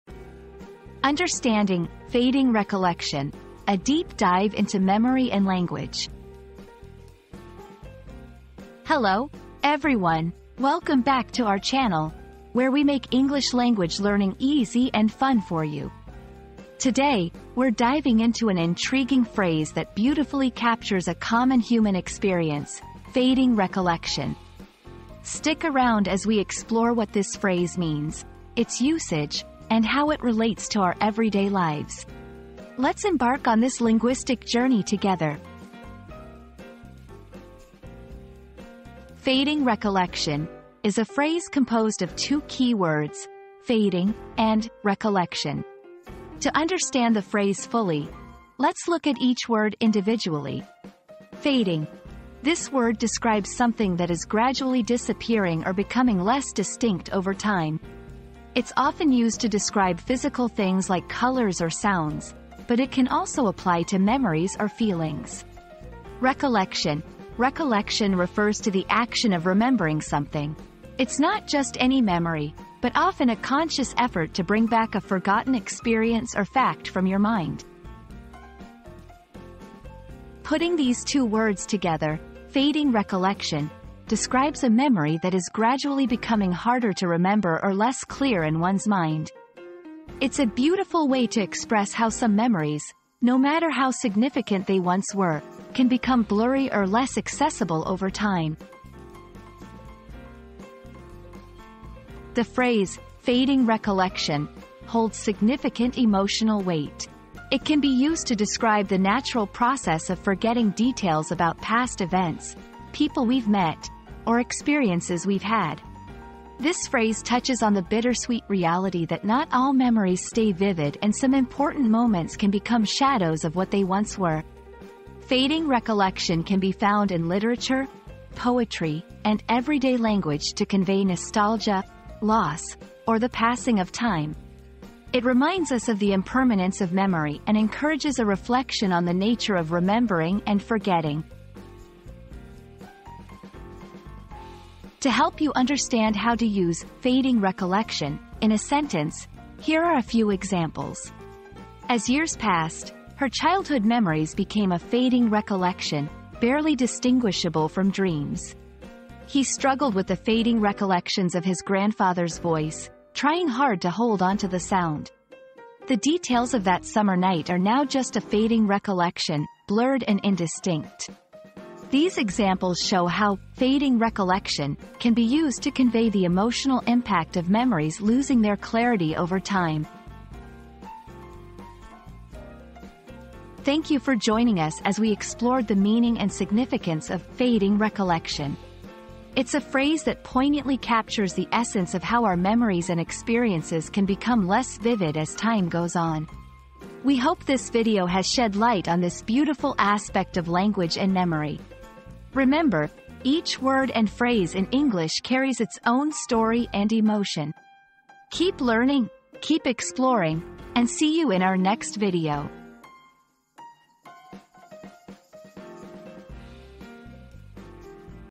Understanding "Fading Recollection": A Deep Dive into Memory and Language
Unraveling Fading Recollection: Memory and Language Explained • Discover the intricate relationship between memory and language in this insightful exploration of 'Fading Recollection'. Dive deep into the complexities of how our memories shape our language and vice versa.

00:00 • Introduction - Understanding "Fading Recollection": A Deep Dive into Memory and Language
00:38 • Breaking Down the Phrase
01:41 • The Significance of "Fading Recollection"
02:24 • Usage Examples in Sentences